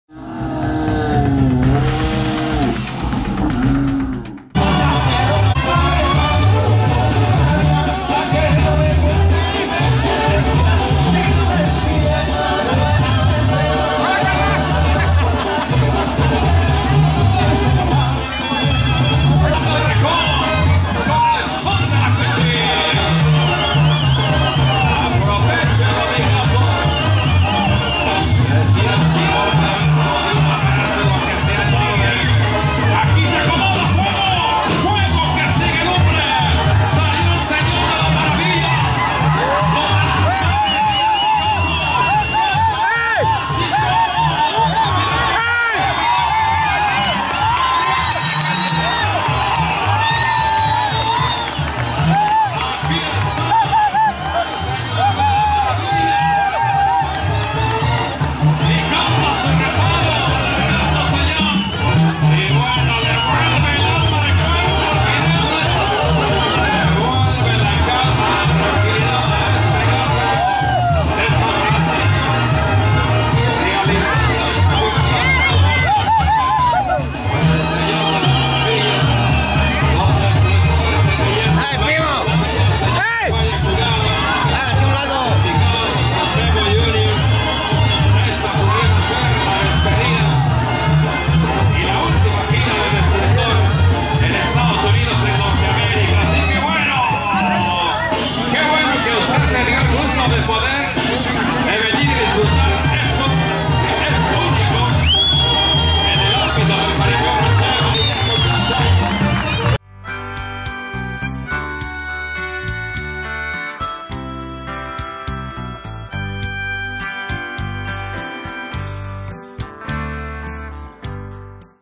SEÑOR DE LAS MARAVILLAS CON GATO DE SAN FRANCISCO OAXACA EN PHILADELPHIA GIRA USA 2018
SENIOR DE LAS MARAVILLAS CON GATO DE SAN FRANCISCO OAXACA EN PHILADELPHIA GIRA USA 2018